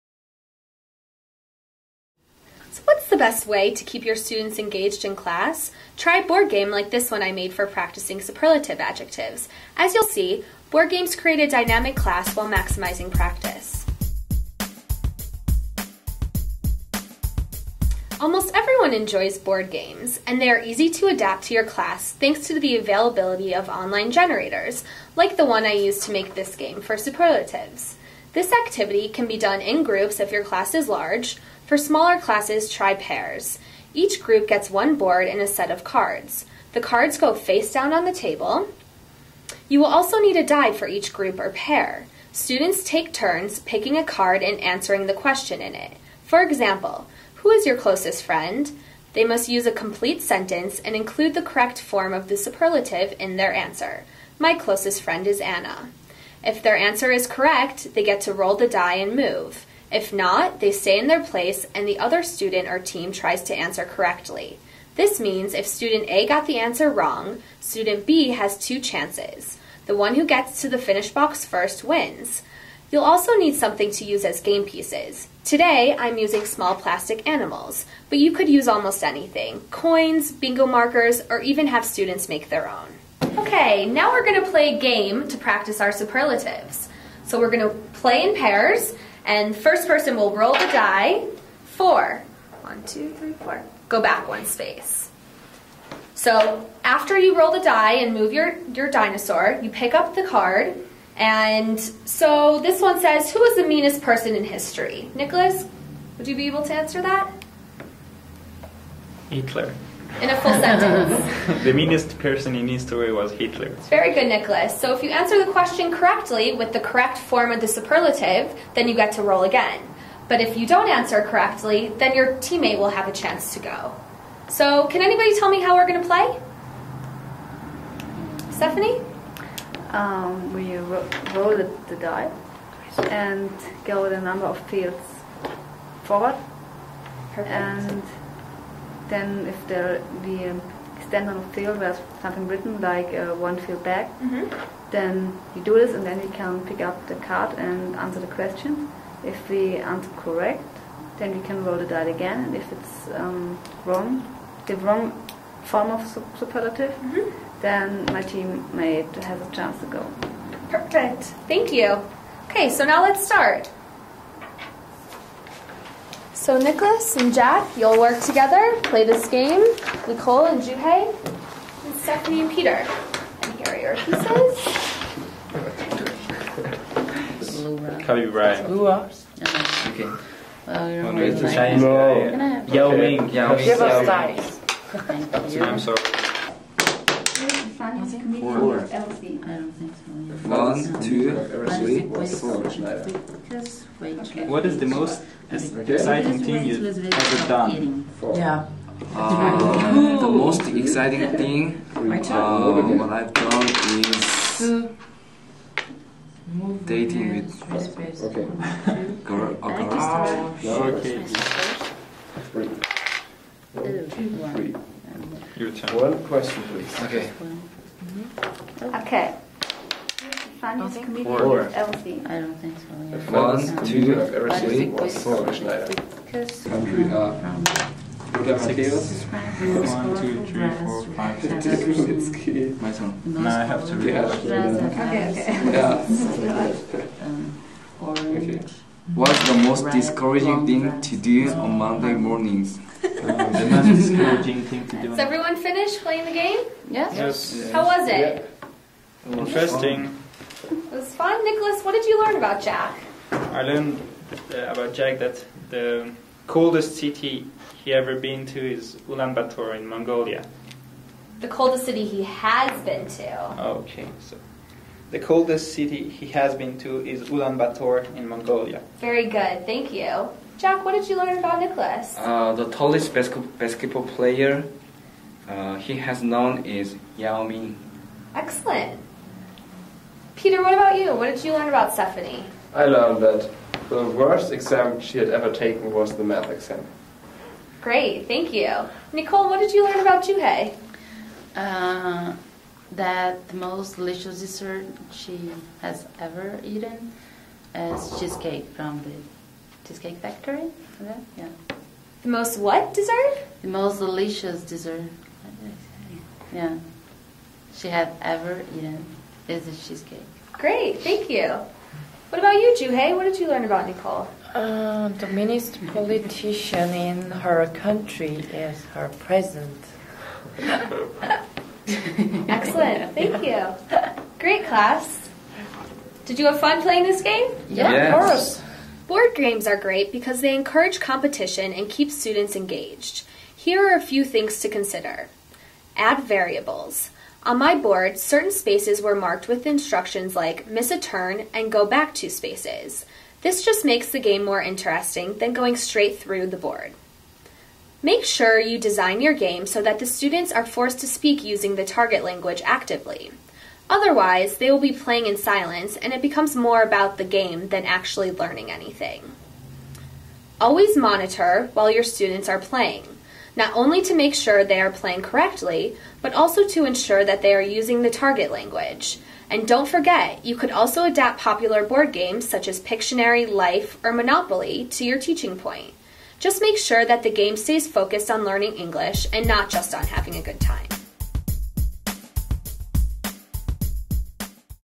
TEFL LIKE A PRO: Teaching Superlatives
Learn more about teaching English as a foreign language and earning TEFL / TESOL certification

"EFL Games and Activities: You can make a simple board game to encourage practice of English as a foreign language. In this TEFL video, EFL students make statements that include the superlative forms of adjectives. The teacher designed this game quickly, using an online template.